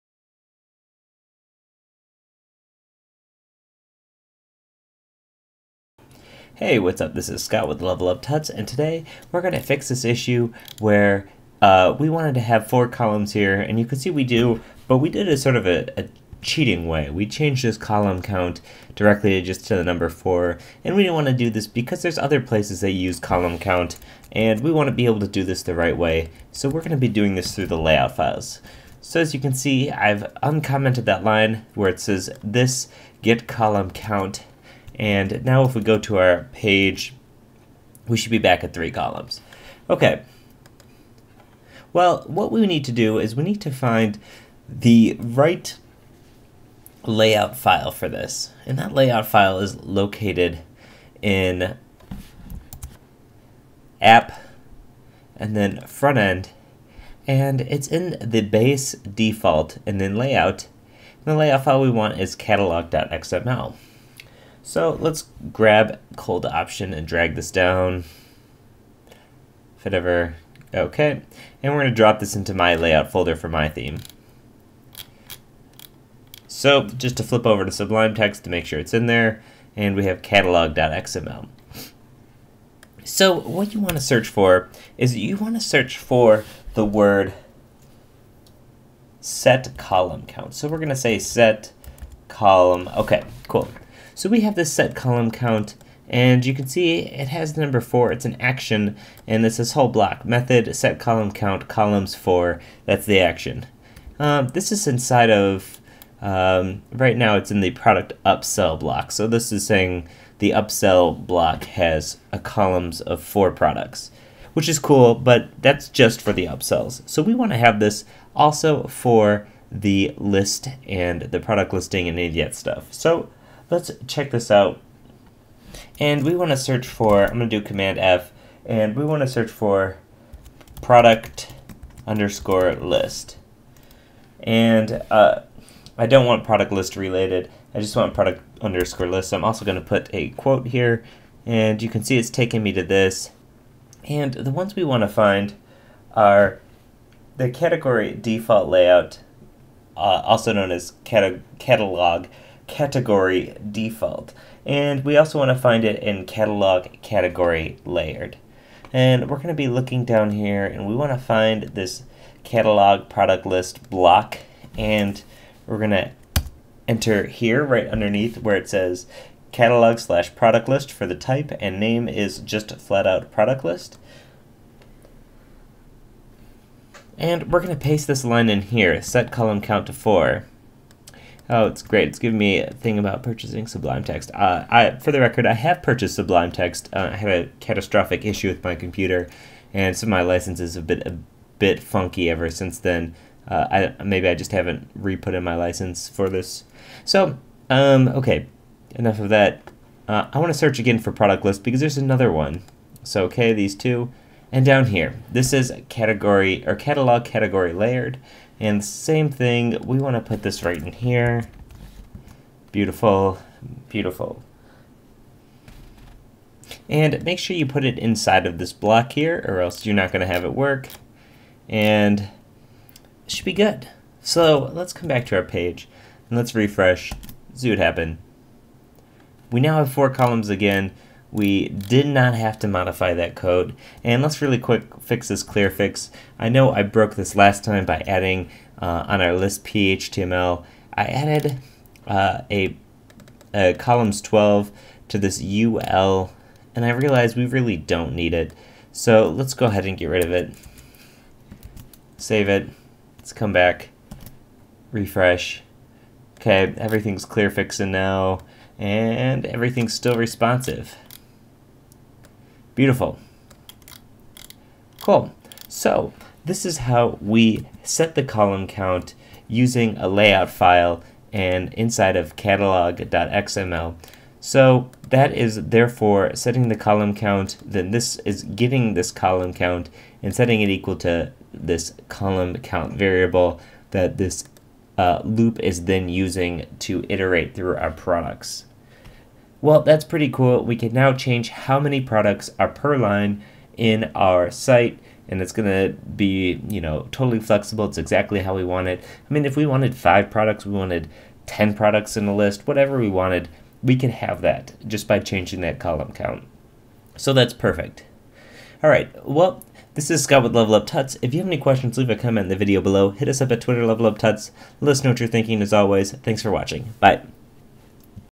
Theming Magento Community #25 - Changing The Product Grid Column Count
The 61st tutorial for Magento Commerce Community Edition. This tutorial shows you how to change the column count in the product grid.

The flexible, open source commerce platform that powers over 250,000 online stores worldwide